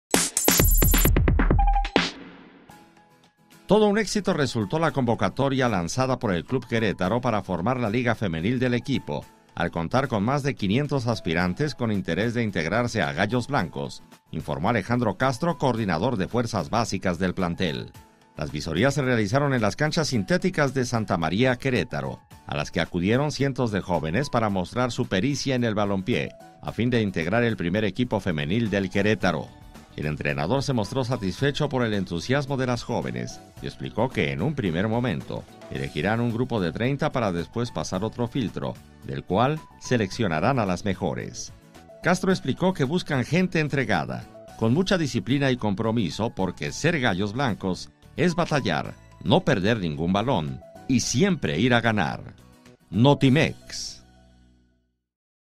Querétaro busca formar su equipo femenil
Más de 500 aspirantes se dieron cita en las instalaciones del club a fin de formar parte de Gallos Blancos